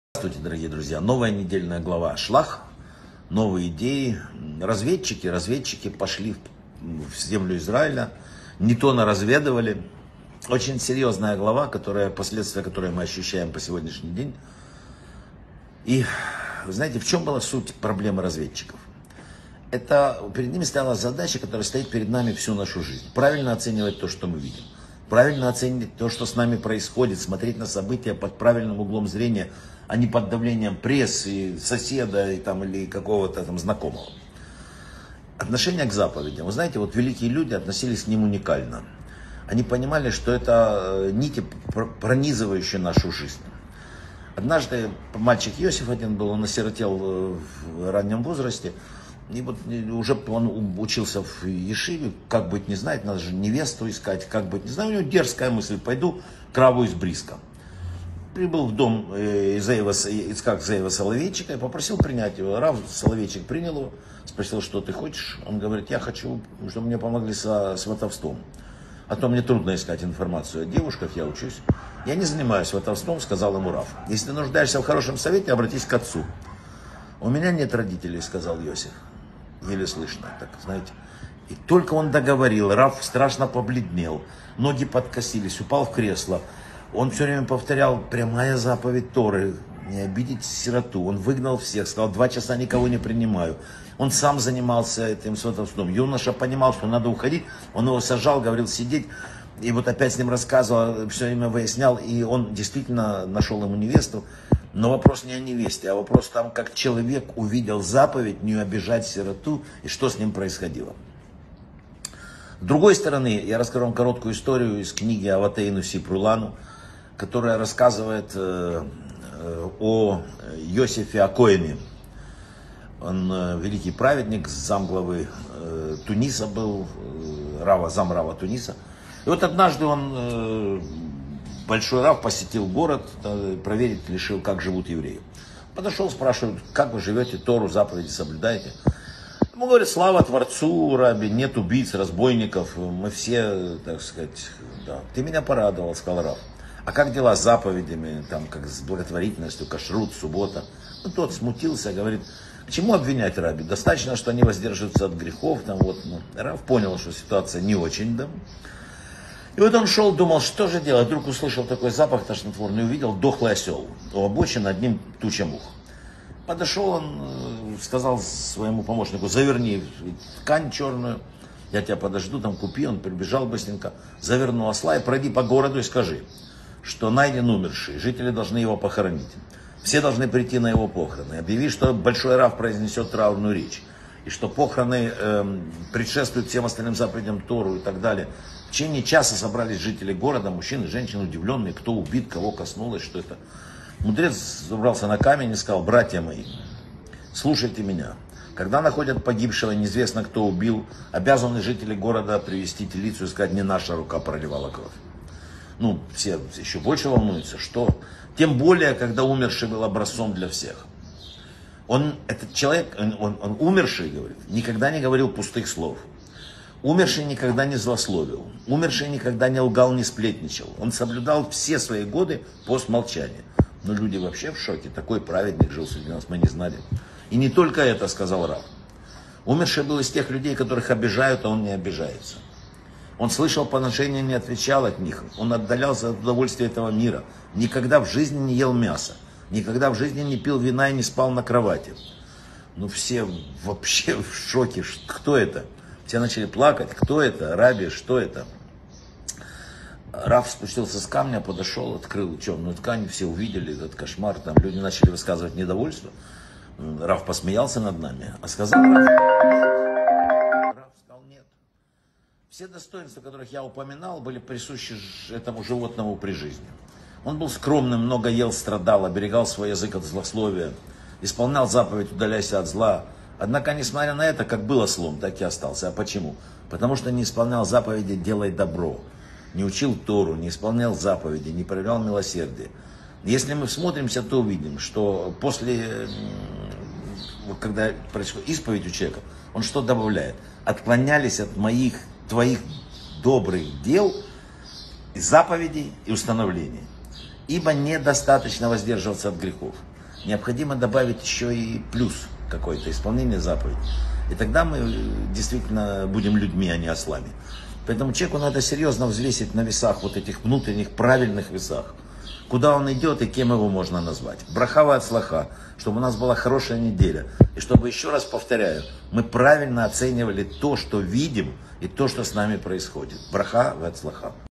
📘 Человек должен правильно оценивать все, что видит. Недельная глава Шлах - Урок 1 | Вадим Рабинович
Чему учит каждого человека произошедшее с разведчиками?
Какая задача стоит перед нами каждый день?
Как великие люди относились к заповедям? История о юноше, пришедшем к раву Ицхаку Зееву Соловейчику за помощью в шидухе.
Какой урок вынесли жители города, не соблюдавшие заповедей, из похорон осла?